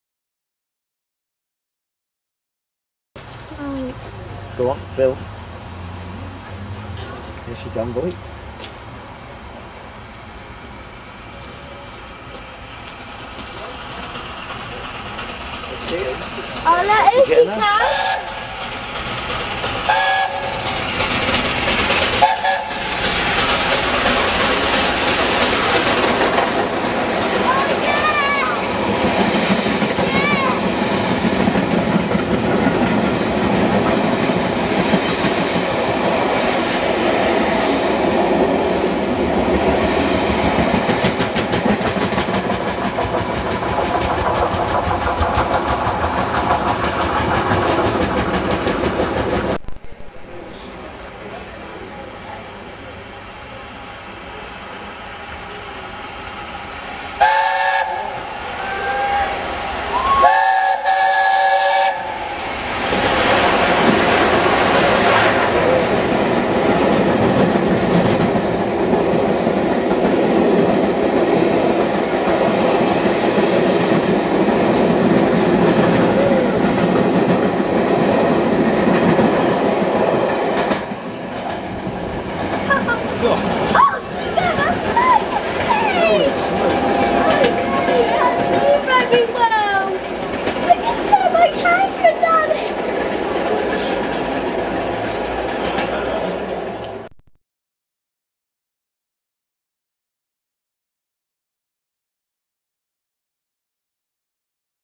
60007 Through Wymondham
Yes,I know on the vid it says 90007, realised too late, must swat up on kettles !! A nice change at our local station with 60007 hauling a charter to Norwich, then later on the return filling the station with the most incredible mix of steam and smoke, wish we could have bottled it, wonderfull, like turning back time !!